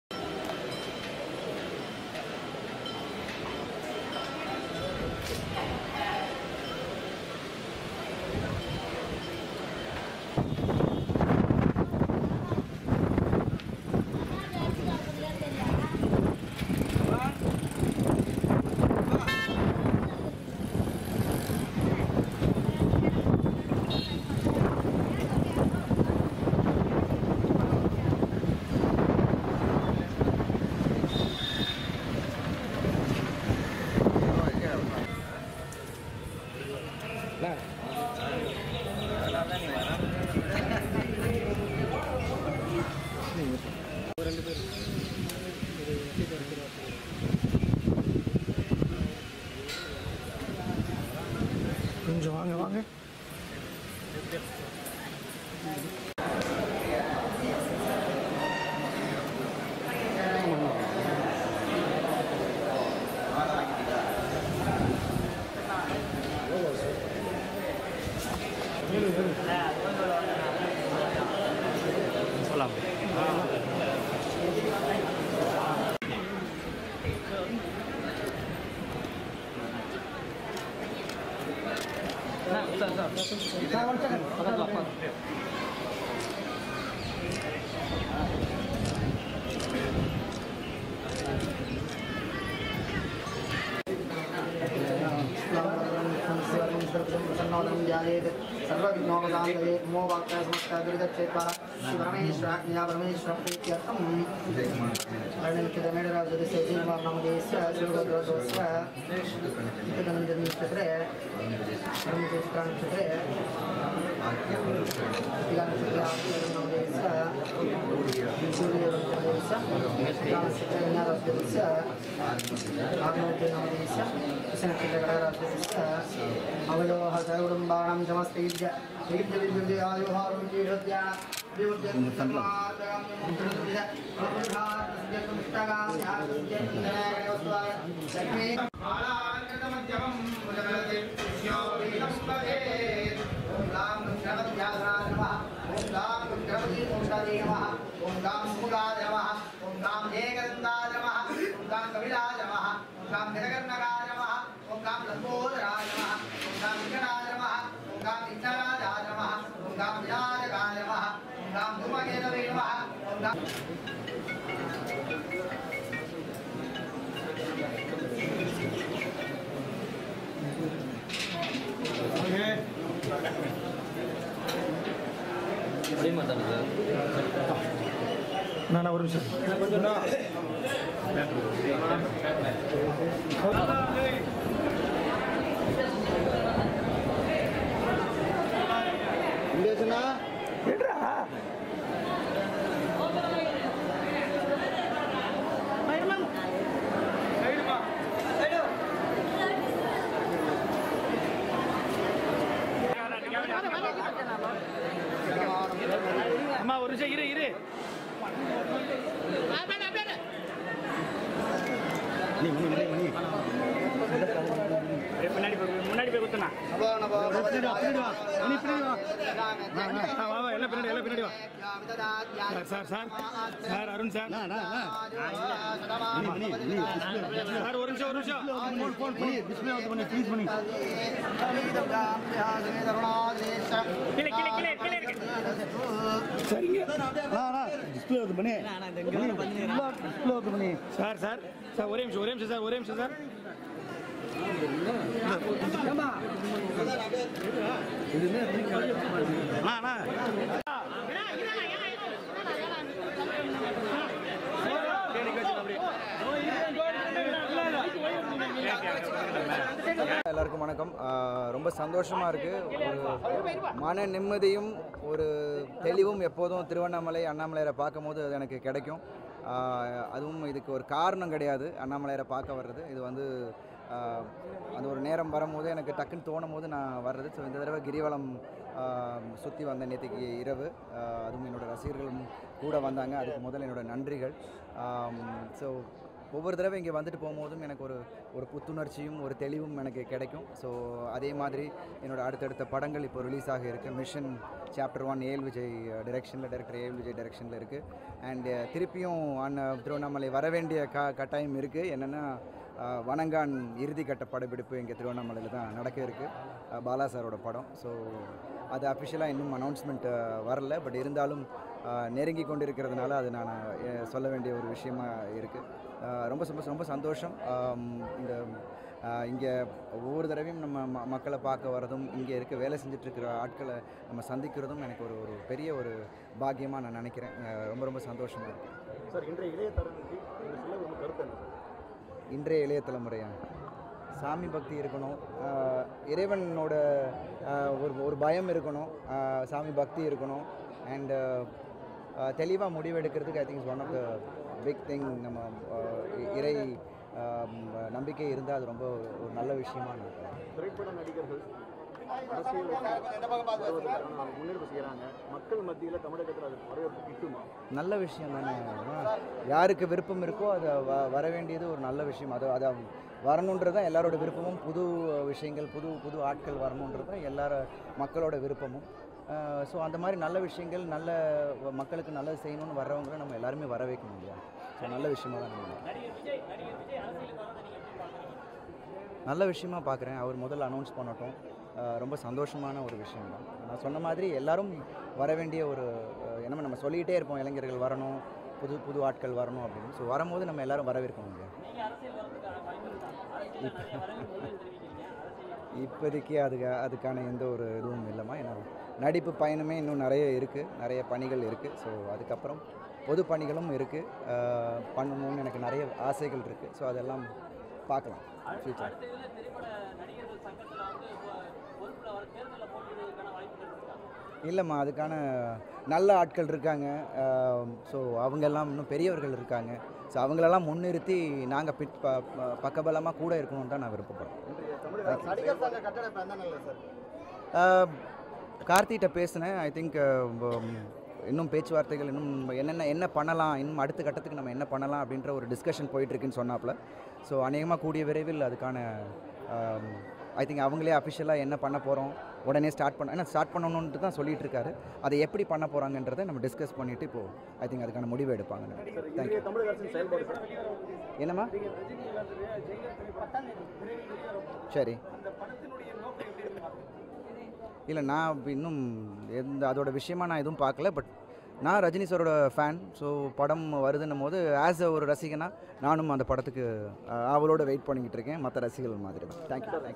Arun Vijay Sami Dharisanam Annamalaiyar Koil
Arun Vijay Sami Dharisanam Annamalaiyar Koil | Arun Vijay

நடிகர் விஜய் அரசியலுக்கு வருவதை வரவேற்பதாக நடிகர் அருண் விஜய் திருவண்ணாமலையில்பேட்டி-மக்களுக்கு நல்லது செய்ய வேண்டும் என்ற எண்ணத்தில் வருபவர்களை நாம் அனைவரும் வரவேற்க வேண்டும். நடிகர் அருண் விஜய் பேட்டி.

திருவண்ணாமலை அண்ணாமலையார் கோவிலில் நடிகர் அருண் விஜய் மற்றும் அவரது மனைவி ஆகியோர் இன்று சாமி தரிசனம் செய்தனர்.

அண்ணாமலையாரை  வேண்டி மாலை அணிந்து விரதம் இருந்து நேற்று இரவு கிரிவலம் வந்த நடிகர் அருண் விஜய் இன்று திருவண்ணாமலை அண்ணாமலையார் கோவிலில்  சம்பந்த விநாயகர், அண்ணாமலையார், உண்ணாமுலையம்மன் ஆகியோரை தரிசனம் செய்தார். அவருக்கு திருக்கோவில் சார்பில் மரியாதை செய்யப்பட்டது.

அதனைத் தொடர்ந்து நடிகர் அருண் விஜய் செய்தியாளர்களை சந்தித்து பேசுகையில் திருவண்ணாமலைக்கு வந்து
அண்ணாமலையாரை  தரிசனம் செய்யும்போதெல்லாம் மன நிம்மதியும் தெளிவும் சந்தோசமும் தனக்கு கிடைப்பதாகவும், அண்ணாமலையாரை தரிசிப்பதற்கு எவ்வித காரணமும் இல்லை, நேற்று இரவு ரசிகர்களோடு கிரிவலம் வந்ததாகவும் திருவண்ணாமலைக்கு வந்து செல்லும்போதெல்லாம் ஒரு புத்துணர்ச்சி கிடைப்பதாக தெரிவித்தவர்.

தொடர்ந்து பேசிய அருண் விஜய் மிஷன் திரைப்படம் விரைவில் வெளியாக உள்ளதாகவும், பாலா தயாரிப்பில் படப்பிடிப்பு நிறைவு ஆகும் தருவாயில் உள்ள வணங்கான் இறுதி கட்ட படப்பிடிப்பு விரைவில் திருவண்ணாமலையில் நடைபெற உள்ளதாகவும் தெரிவித்தார்.

திருவண்ணாமலை மக்கள் மற்றும் அருணாச்சலேஸ்வரர் திருக்கோவிலில் பணியாற்றுபவர்களை சந்திப்பதில் பெருமகிழ்ச்சி அடைவதாகவும் இளம் தலைமுறைகள் சாமி பக்தியும் இறைவனிடம் ஒரு அச்சமும் தெளிவான முடிவு எடுக்கும் அளவிற்கு இறைவன் மீது நம்பிக்கை இருக்க வேண்டும் என்றும், திரைப்பட நடிகர்கள் அரசியலுக்கு வருவது நல்ல விஷயம் என்றும் மக்களுக்கு நல்லது செய்ய வேண்டும் என்ற எண்ணத்தில் வருபவர்களை நாம் அனைவரும் வரவேற்க வேண்டும் என்றும் தெரிவித்தார்.

நடிகர் விஜய் அரசியலுக்கு வருவது நல்ல விஷயமாக தான் பார்ப்பதாகவும் அவர் முதலில் அறிவிக்க வேண்டும் இளைஞர்களையும் புதியதாக வருபவர்களையும் நாம் அனைவரும் ஒன்றிணைந்து வரவேற்க வேண்டும் என்றும் தான் அரசியலுக்கு வருவது சாத்தியமில்லை என்றும் தனது நடிப்பு பயணம் நிறைய உள்ளதாகவும் அதில் சாதிக்க நிறைய பயணங்களை தான் மேற்கொள்ள வேண்டும் என்றும் பொதுப்பணிகளையும் தான் மேற்கொள்ள இருப்பதாகவும் விரைவில் பார்த்துக் கொள்ளலாம் என்று நகைச்சுவையாக தெரிவித்தார்.

திரைப்பட சங்கத்தில் மூத்தவர்கள் பலர் உள்ளார்கள் இளையவர்கள் தாங்கள் அவர்கள் பின் நின்று பக்கபலமாக இருப்போம் என்றும் நடிகர் சங்க கட்டிடம் குறித்து செய்தியாளர்கள் எழுப்பிய கேள்விக்கு பதில் அளிக்கையில் நடிகர் சங்க கட்டிடம் குறித்து கார்த்திக்கிடம் பேசிய போது  அடுத்த கட்ட நடவடிக்கைகள் மேற்கொள்வது குறித்து ஆலோசித்து வருவதாக தெரிவித்திருப்பதாகவும்.

 விரைவில் நடிகர் சங்க கட்டிடம் குறித்து அதிகாரபூர்வ முடிவை சங்கத்தினர் கலந்தாலோசித்து முடிவெடுப்பார்கள் என்றும் தெரிவித்தார்.

ஜெயிலர் படம் குறித்து கருத்து கேட்ட செய்தியாளர்களின் கேள்விக்கு நானும் ரஜினிகாந்தின் ரசிகன் என்றும் ஜெய்லர் படத்திற்காக தான் ஆவலோடு இருப்பதாகவும் தெரிவித்தார்.